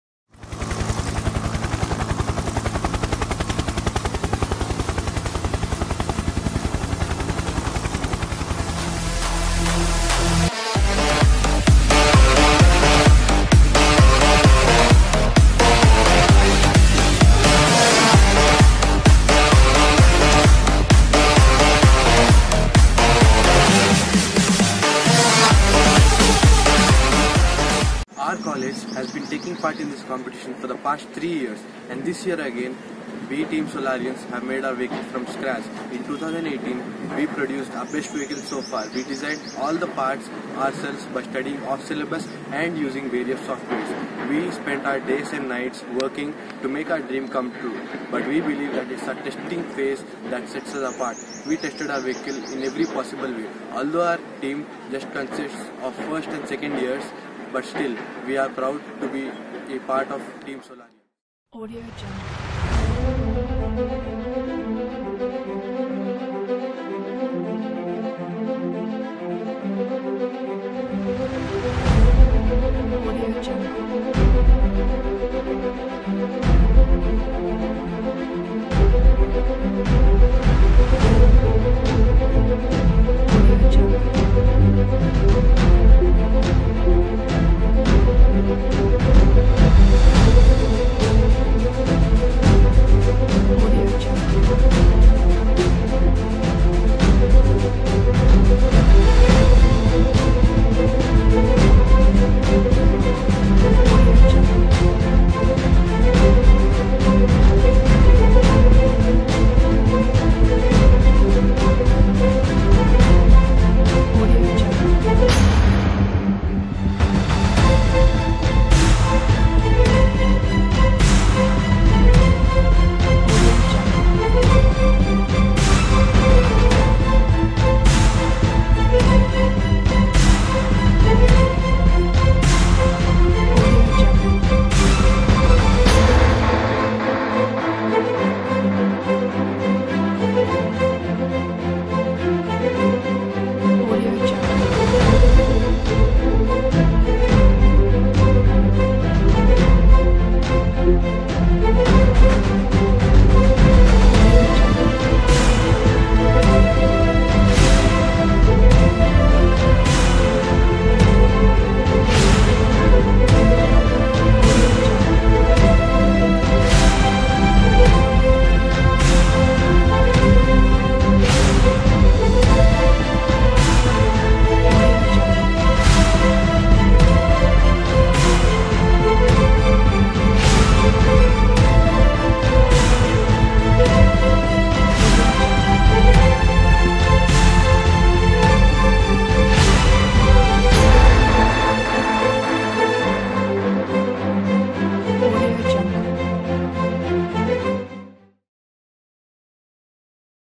ESVC19-A-35 TEAM SOLARIANS 3.0
Asia's Biggest Solar Car Event: Electric Solar Vehicle Championship-ESVC organised by Imperial Society of Innovative Engineers-ISIEINDIA.
Season 6.0, Final Event @ Chandigarh University, Punjab
25th to 31st March 2019